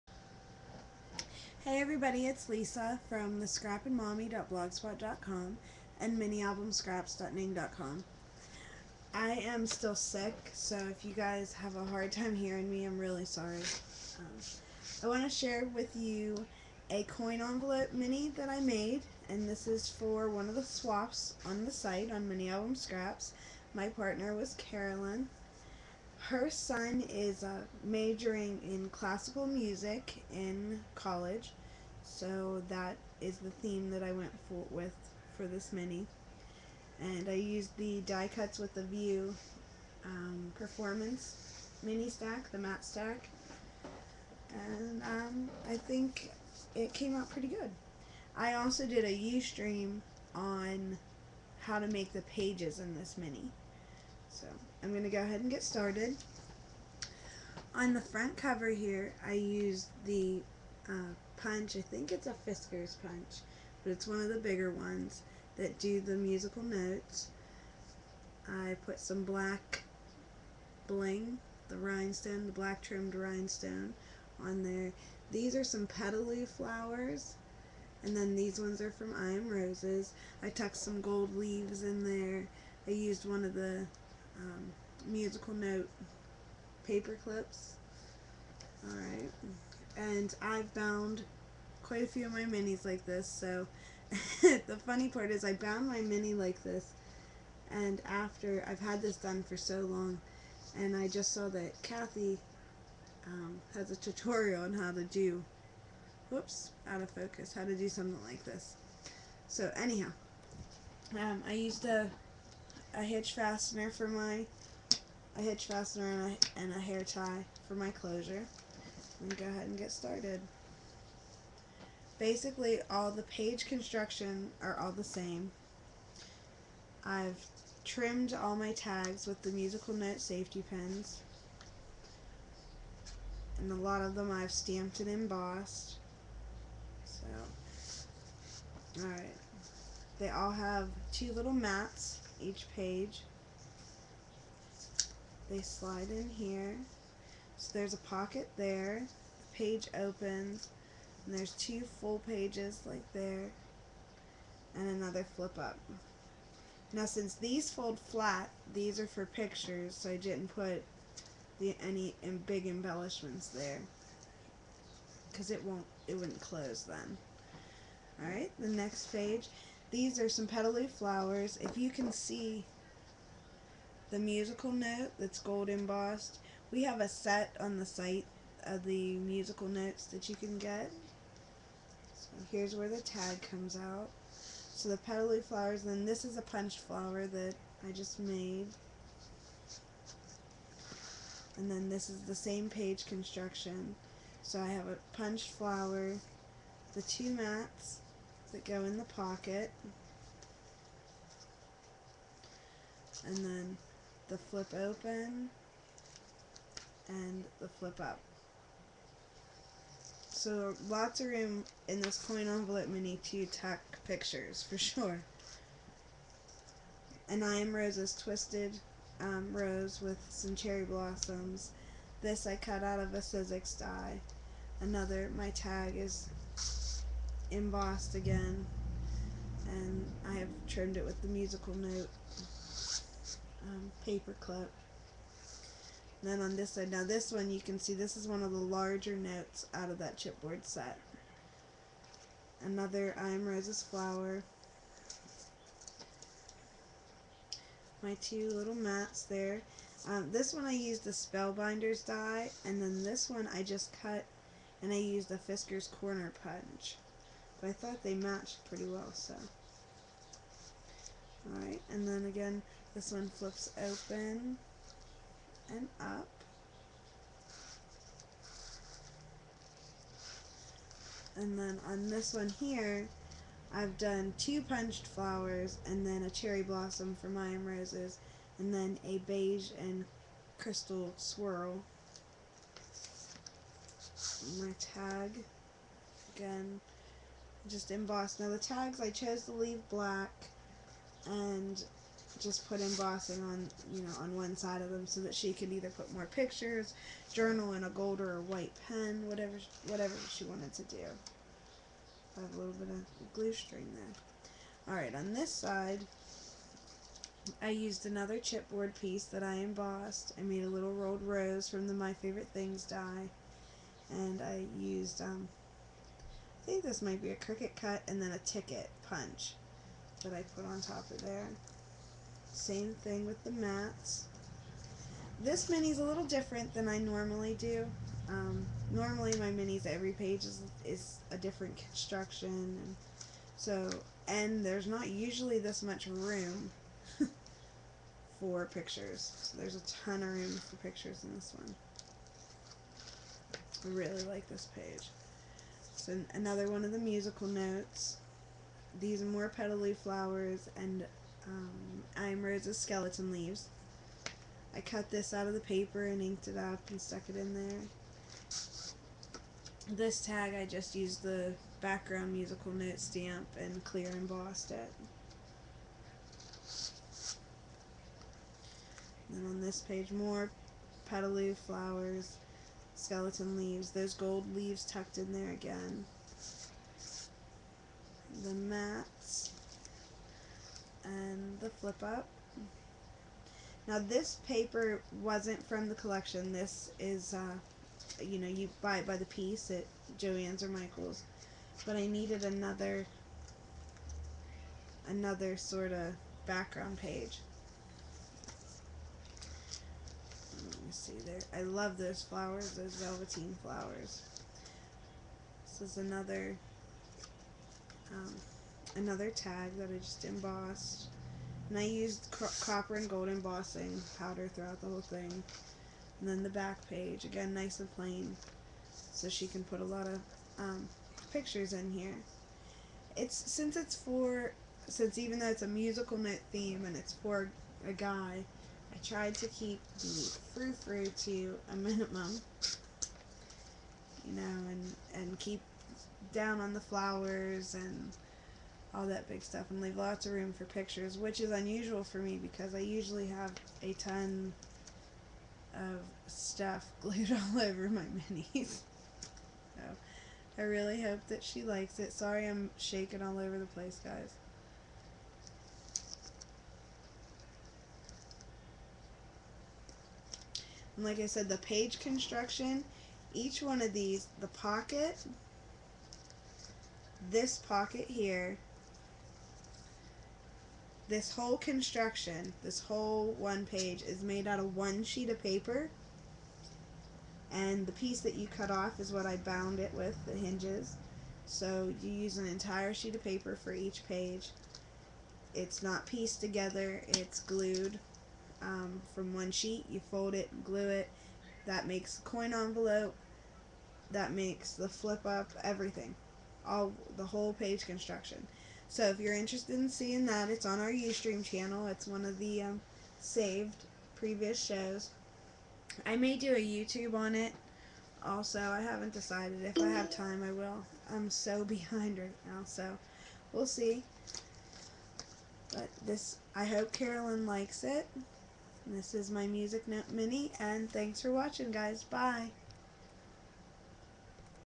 Musical Note Coin Envelope Mini fro Swap at MAS
This coin envie mini is for Carolyn for a swap I am in at minialbumscraps.ning.com!!